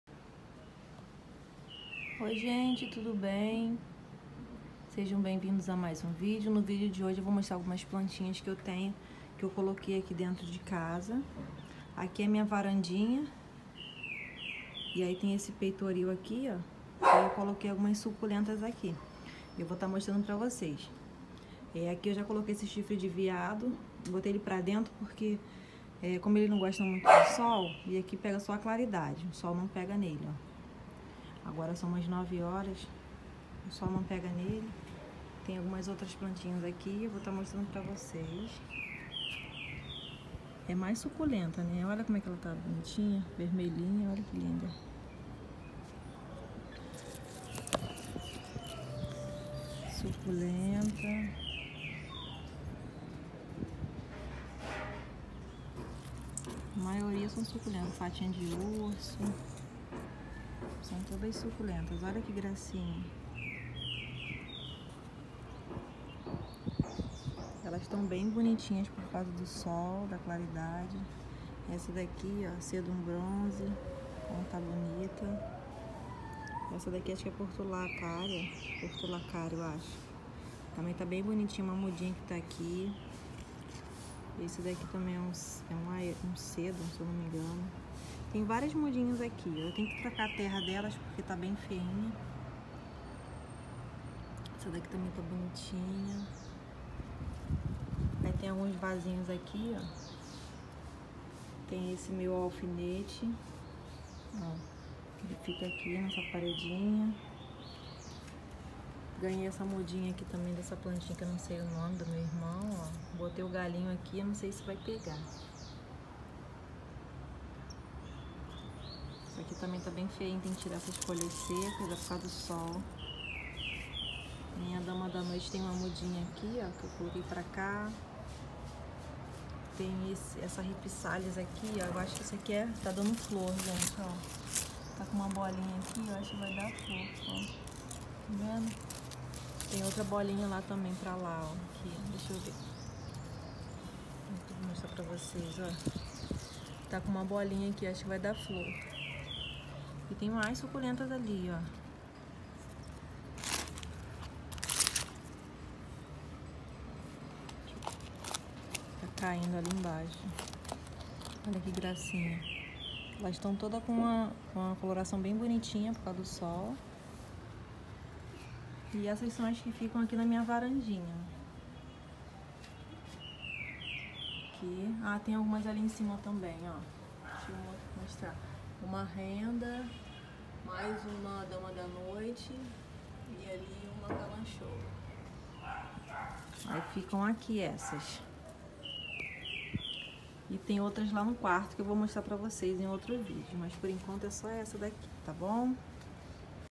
Suculentas em casa , onde colocar. 1/3
Oi Pessoal tudo bem com vocês ,no vídeo de hoje eu vou mostrar onde eu coloquei algumas suculentas dentro de casa ,escolhi o melhor lugar onde pega mais sol pra poder colocar .Espero que vocês gostem se inscrevam no canal ￼.